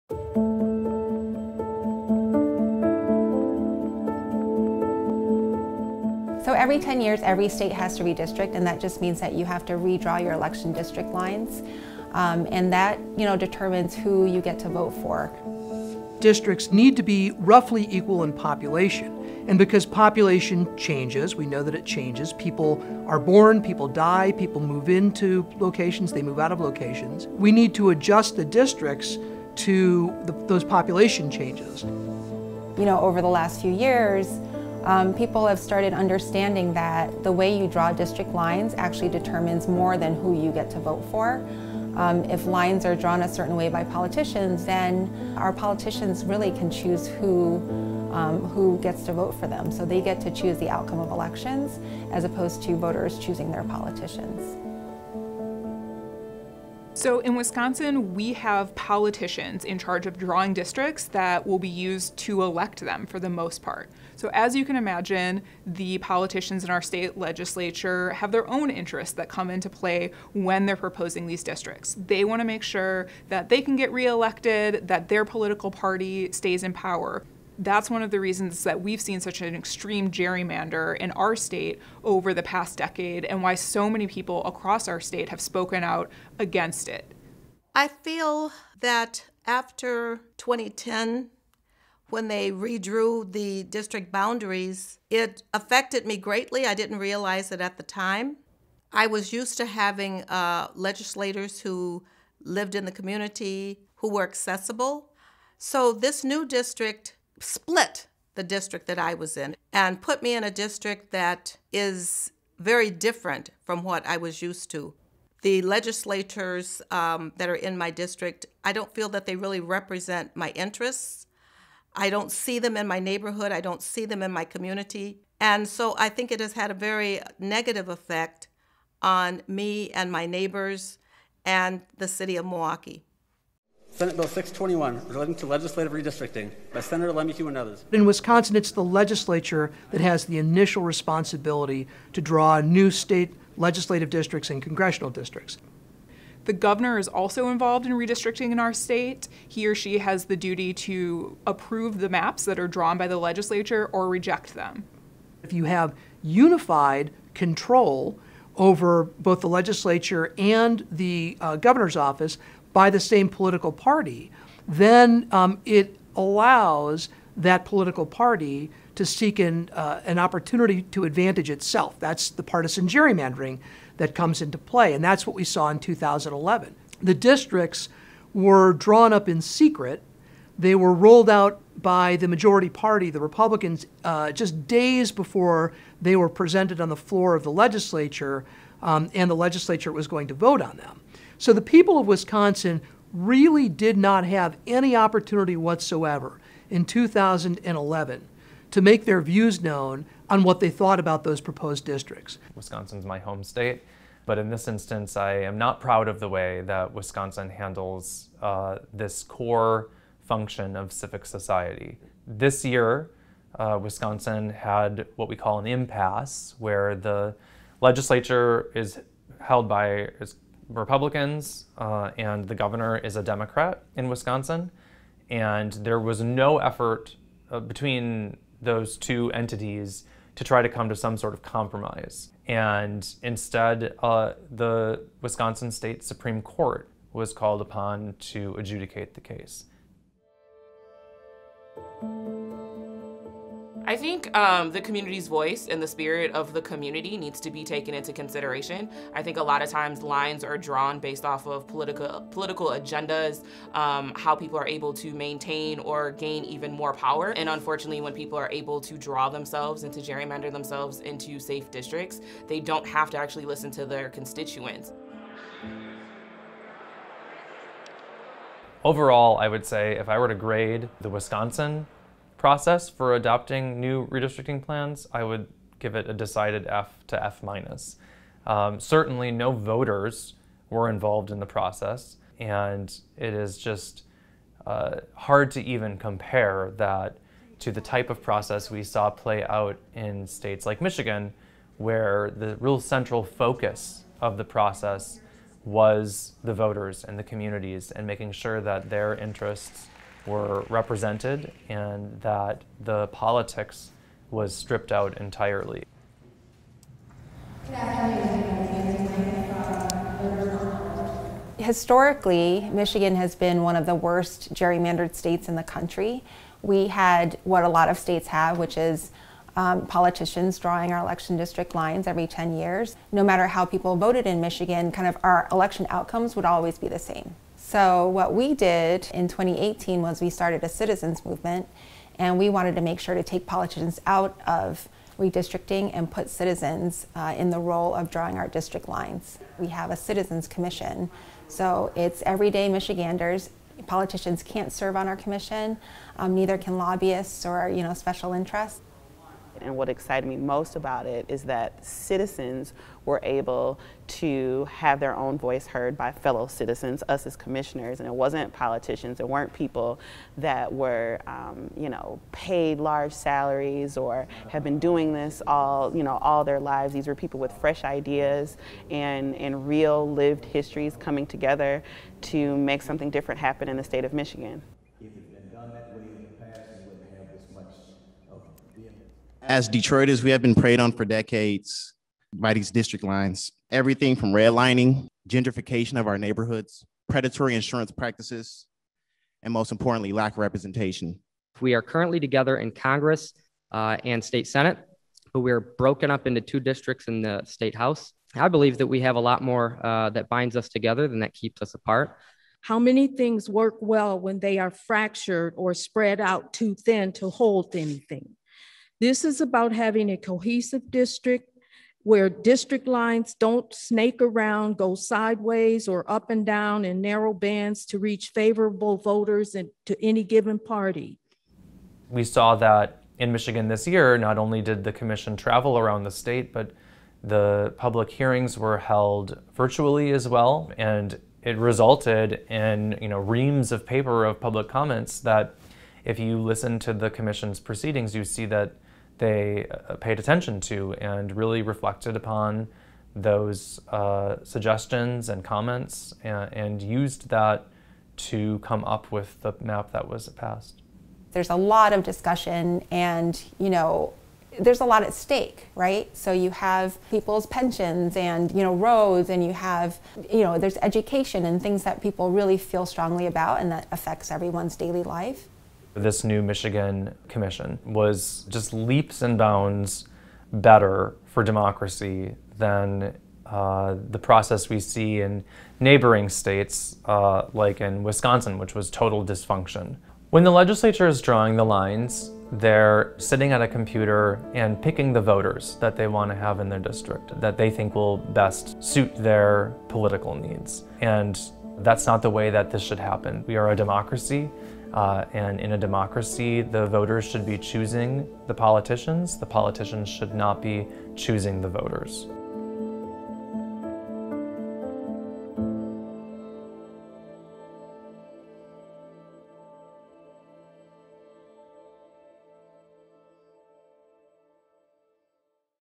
Who Draws the Lines? Fighting for Fair Maps in Wisconsin and Michigan
Every 10 years, every state must redistrict. That means that states redraw election district lines, and that determines who you get to vote for.

This process should be straightforward. Unfortunately, in the U.S., politicians in many states engage in partisan gerrymandering. This means politicians get to pick their voters, instead of voters picking their politicians.

Citizens in both Michigan and Wisconsin share their stories about the redistricting processes in their respective states and discuss what works well and what doesn’t.